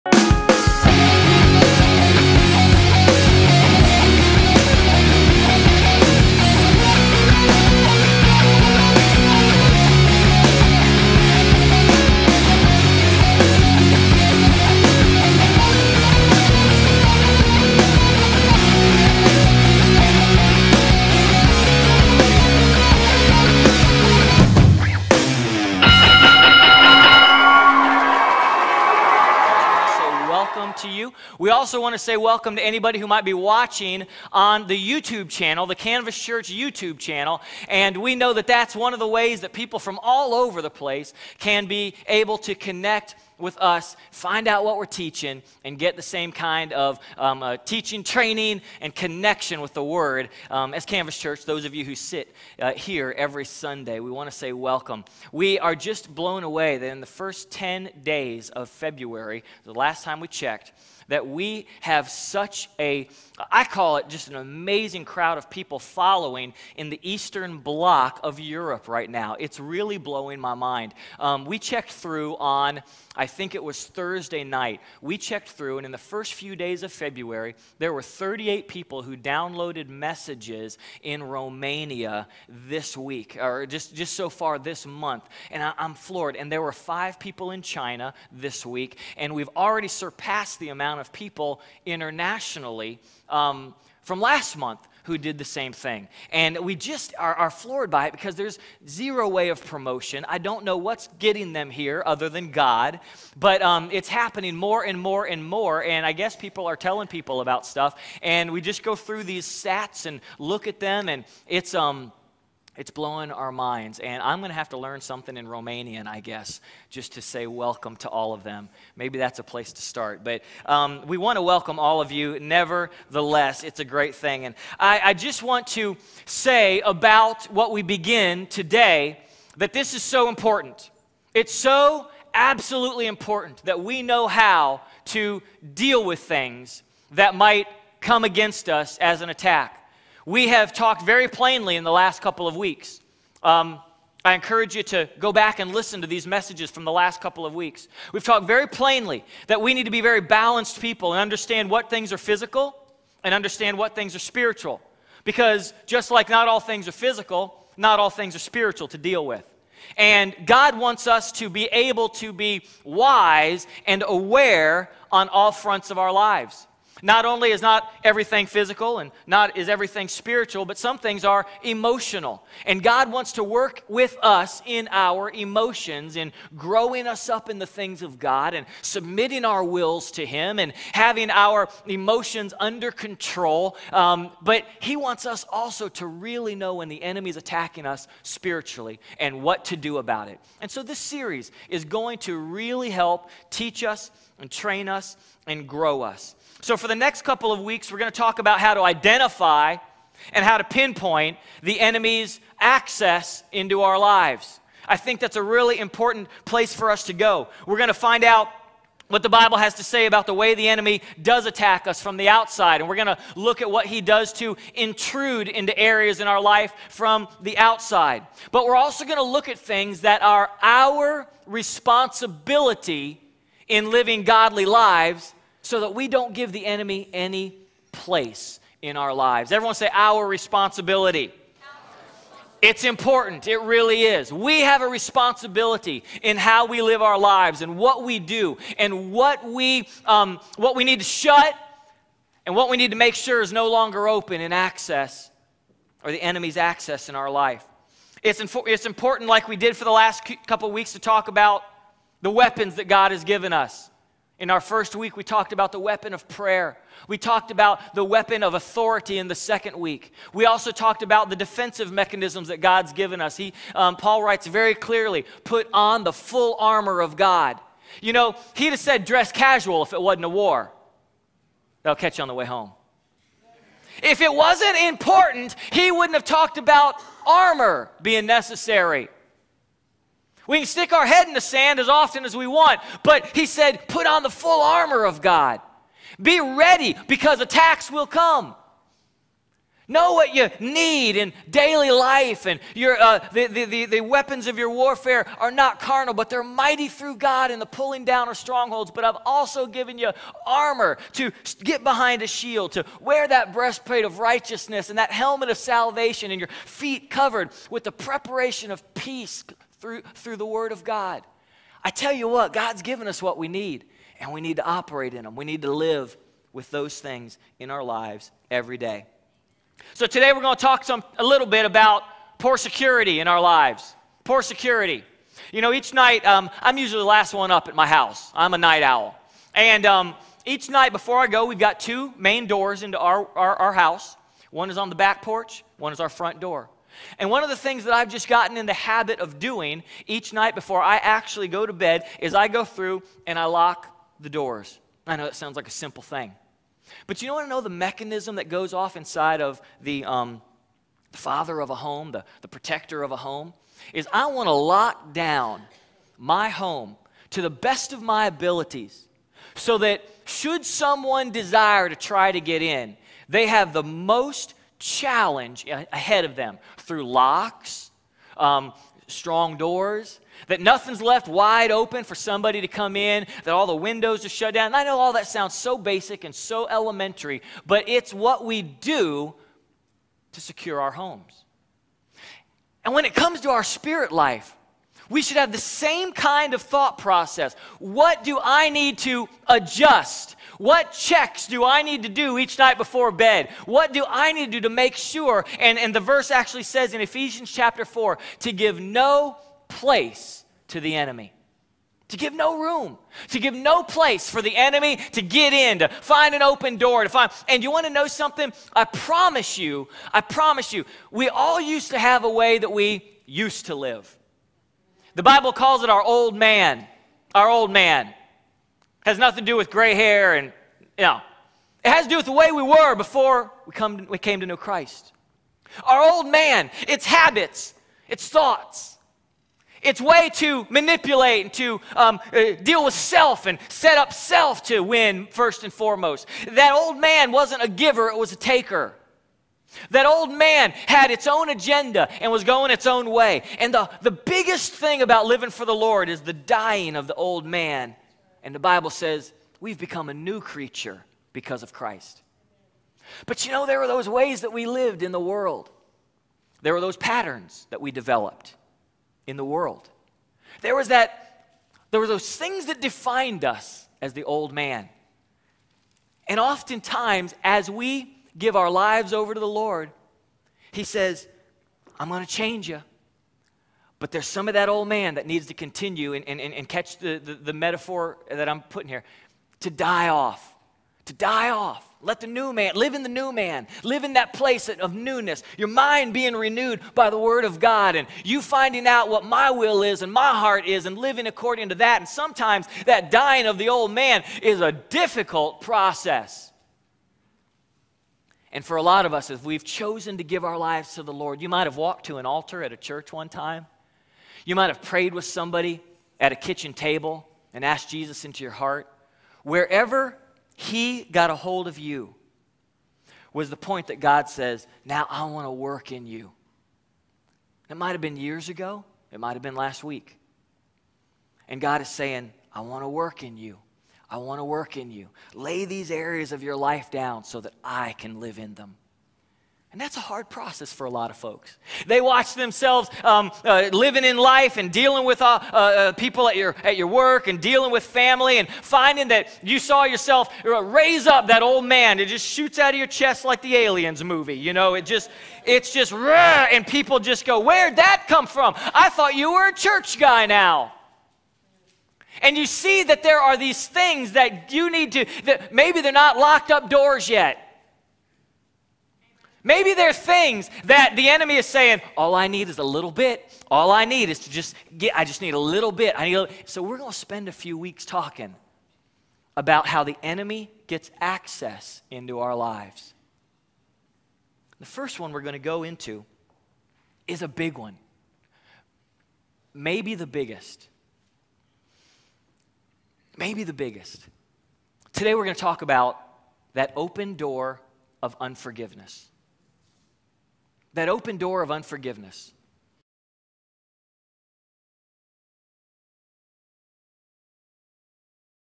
Shadowboxing (week 3) Forgiveness, Part 1
So for the next couple of weeks, we will talk about how to identify and pinpoint the enemys access into our lives.

Going to see what the Bible has to say about the way that the enemy attacks from outside and tries to intrude into our lives as well as what our responsibilities are in living a Godly life so as to NOT give him any room for influence.

It was important to talk about the weapons that we have available to us through Gods Word and the authority we have to stand up against these attacks BEFORE we studied what Scripture has to say about how the enemy tries to attack.